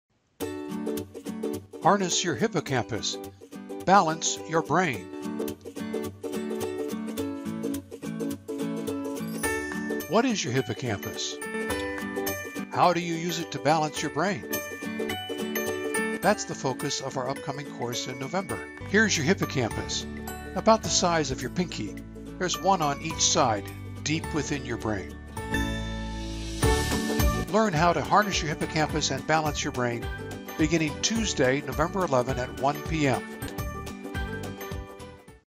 OLLI Fall 2020 Matheson Preview - Harness your Hippocampus!
OLLI Chico has many very exciting courses for the Fall, 2020 semester.  Join us for good, clean fun and BRAIN HEALTH!